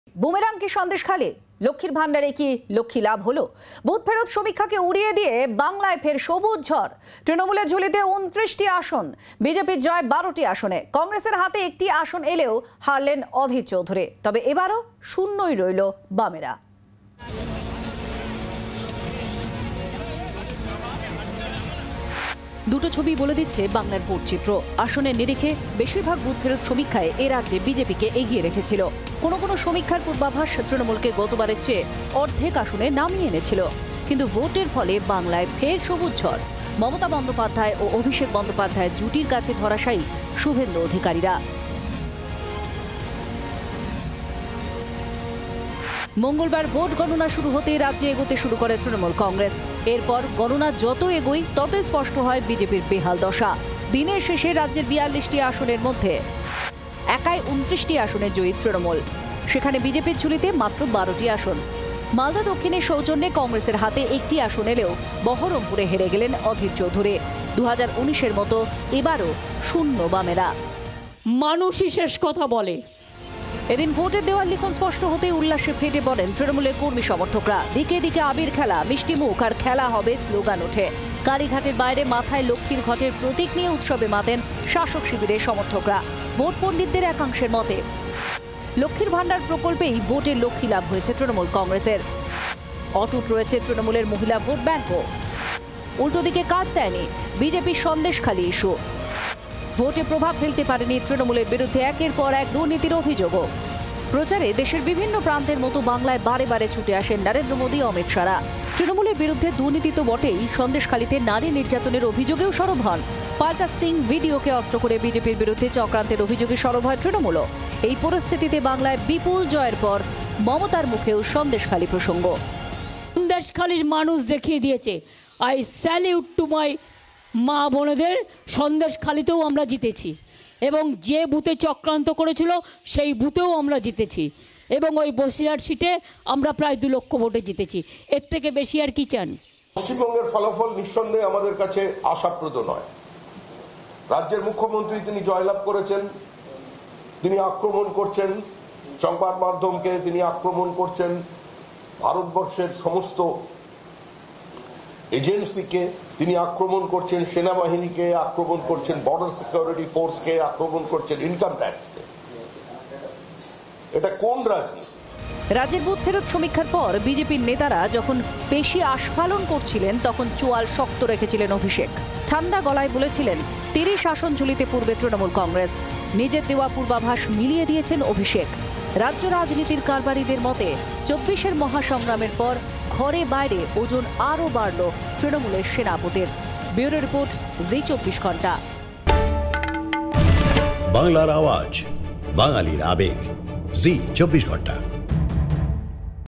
Lok Sabha Election Result: বাংলায় ফের সবুজ ঝাড়, ২৯ আসন তৃণমূলের ঝুলিতে | Zee 24 Ghanta
Lok Sabha Election Result: বাংলায় ফের সবুজ ঝাড়, ২৯ আসন তৃণমূলের ঝুলিতে, দেখুন এই মুহূর্তের টাটকা আপডেট...